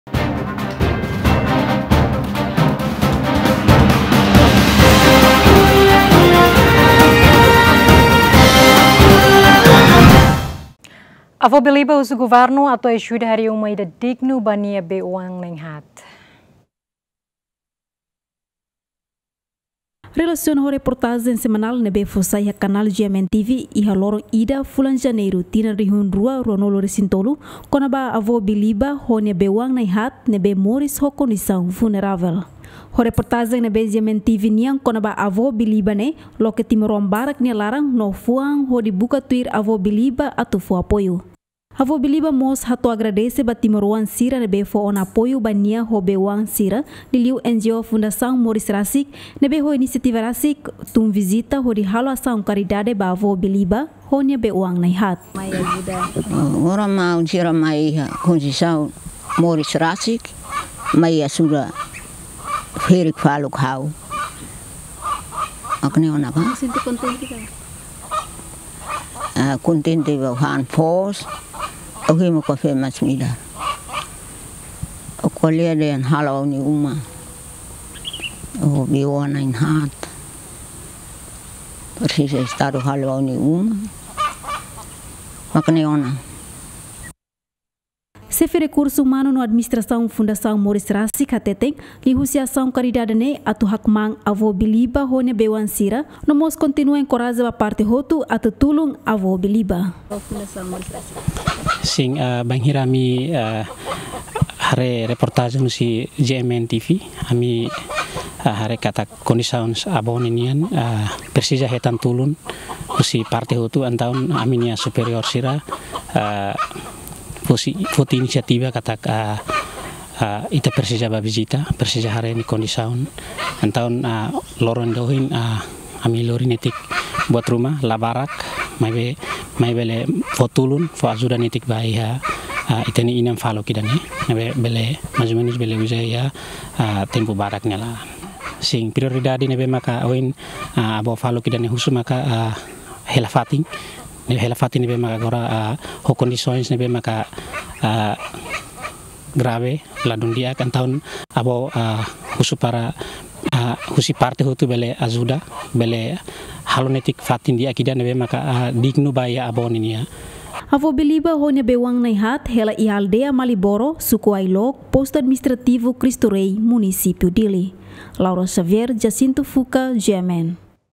Avo BI LIBA husu Governu atu ajuda harii uma ida dignu ba bei-oan nain hat
Avo BI LIBA husu Governu atu ajuda harii uma ida dignu ba nia bei oan nain hat